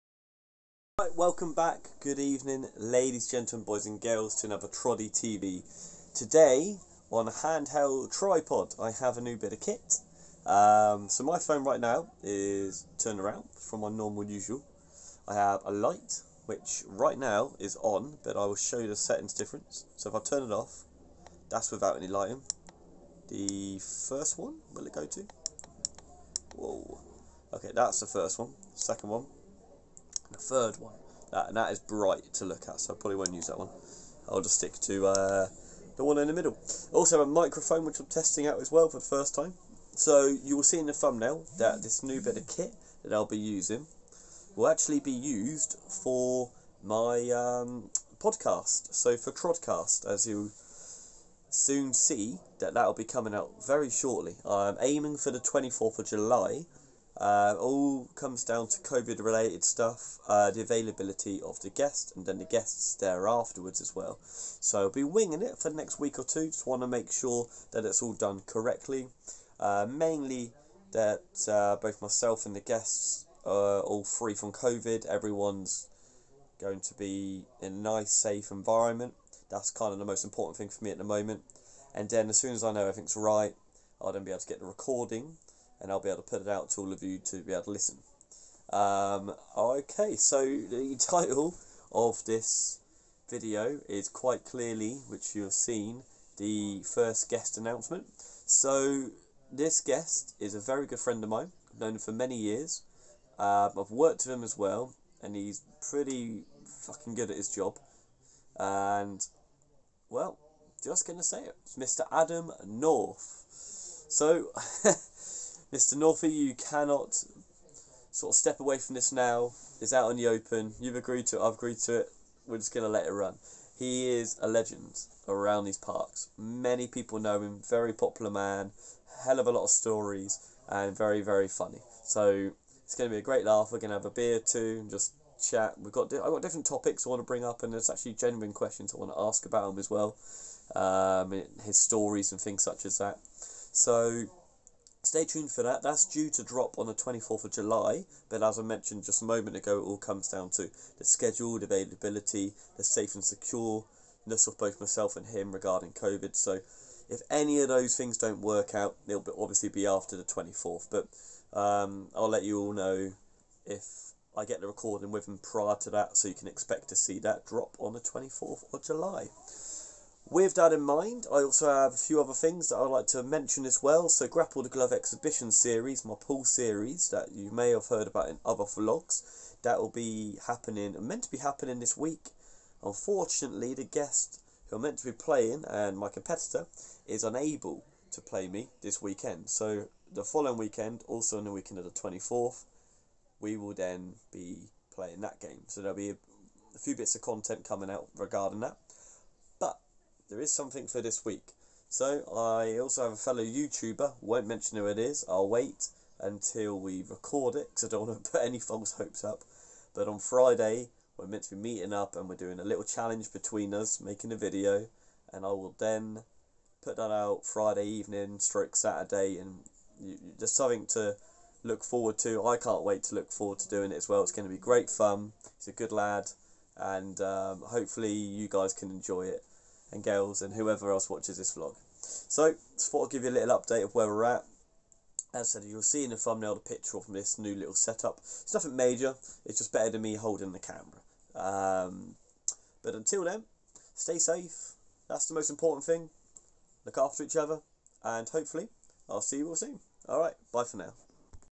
TroddCast 1st Guest Announced! + My New Vlogging Kit 🎥
TroddCast Coming Soon, Vlogging Kit upgraded, and general chit chat!